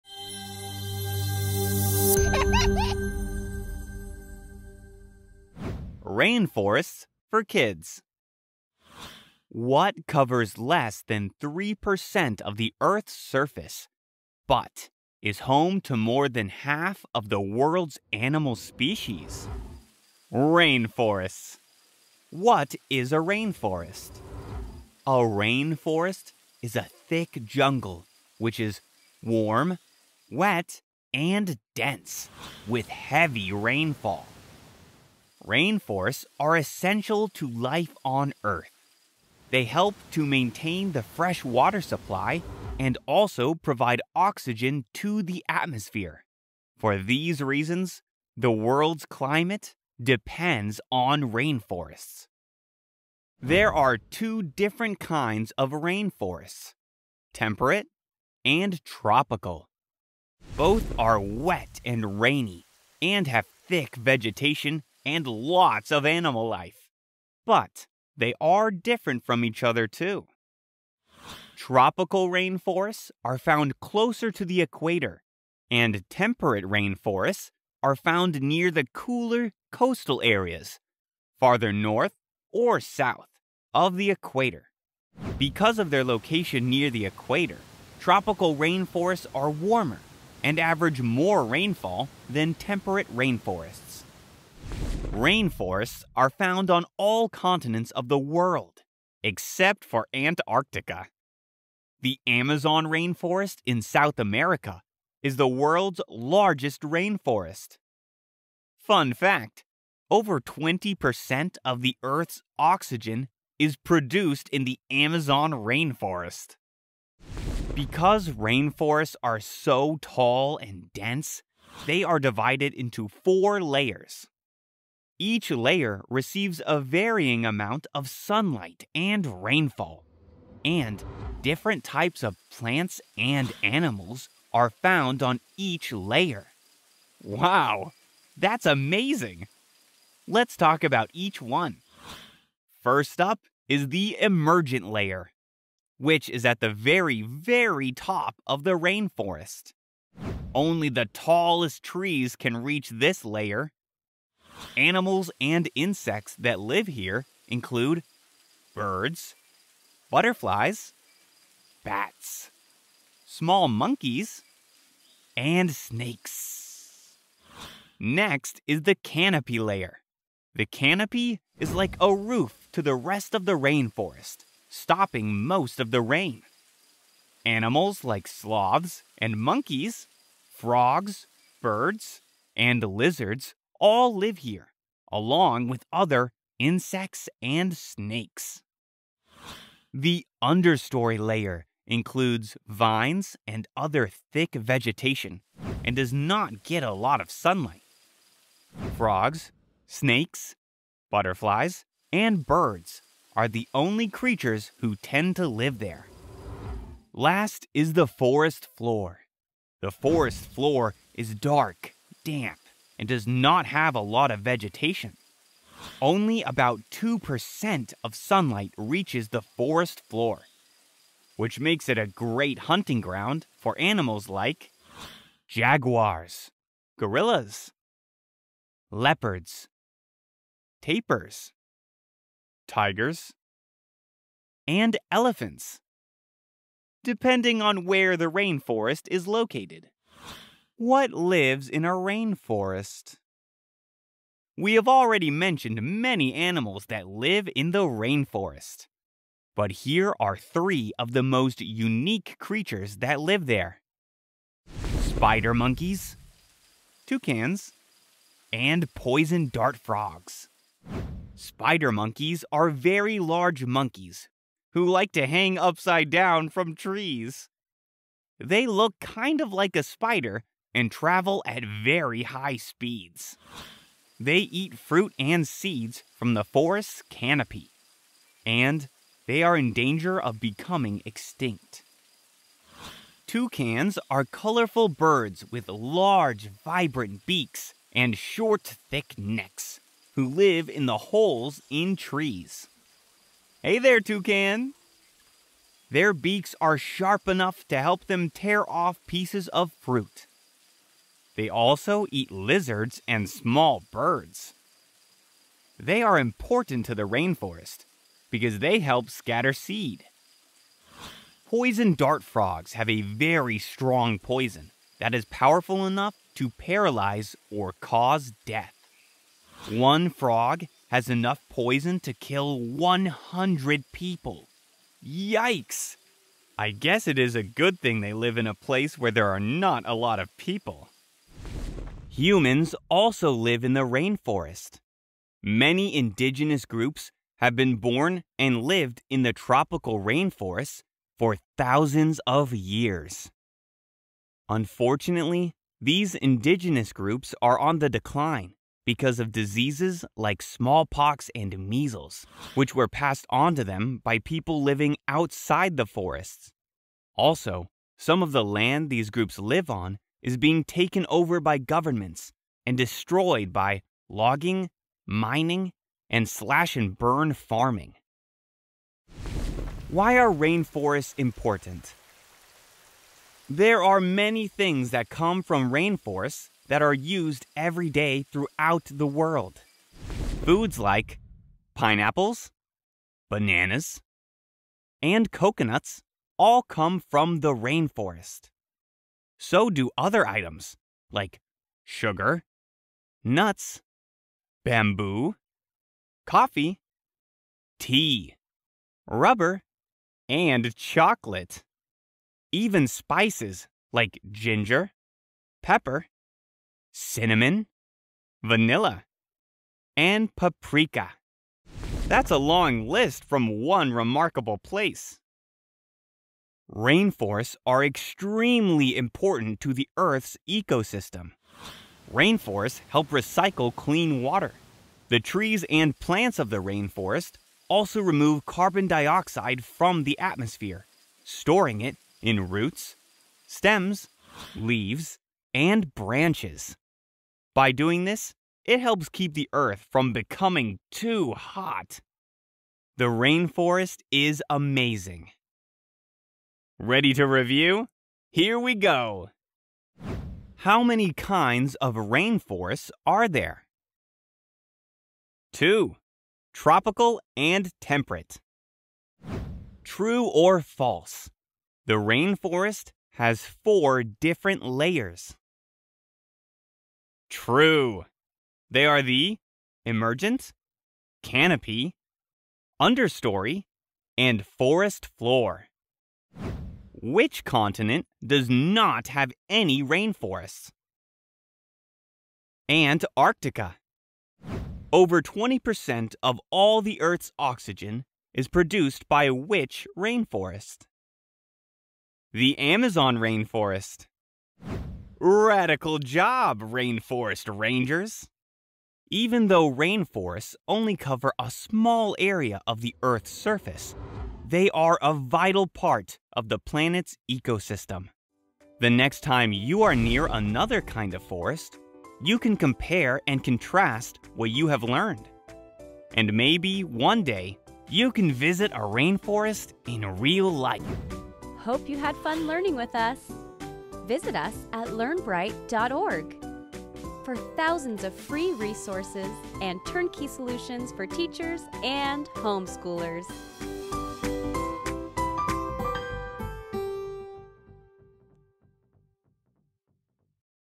Rainforests for Kids | Learn all about the two types of rainforests
Rainforests for Kids explores the world of these dense, wet regions of the world. There are two kinds of rainforests—temperate and tropical. Despite covering less than 3% of the earth's surface, rainforests are home to more than half the world's animal species! They are essential to life on earth because they help maintain the fresh water supply and provide oxygen to the atmosphere. The world's climate depends on rainforests!

When you compare temperate and tropical rainforests, you will find a few differences. Tropical rainforests are closer to the equator and are thus a little warmer and have more rainfall. Temperate ones are found in cooler coastal areas. You can find a rainforest in every continent except for Antarctica. The largest rainforest in the world is the Amazon rainforest. Over 20% of the earth's oxygen is produced by the vegetation there!

There are four layers to a rainforest: emergent, canopy, understory, and forest floor. Different animals live in different layers. Small monkeys and snakes, for instance, live at the top. At the bottom, you would find leopards, elephants, and gorillas. The video describes some interesting facts about three cool animals in particular. Keep watching to learn more!

We hope you and your student(s) enjoyed learning about these amazing forests! If you want to learn even more, head over to our website and download one of our many free lesson plans about rainforests, full of activities, worksheets, and more!

What you will learn in Rainforests for Kids:
0:00 Introduction to rainforests
0:53 Two kinds of rainforests: temperate and tropical
1:41 The Amazon rainforest is world's largest rainforest
2:22 Emergent and canopy layers
3:09 Understory and forest floor layers
4:00 What lives in a rainforest?
5:31 Humans in the rainforest
6:09 Why rainforests are important
7:26 Review of the facts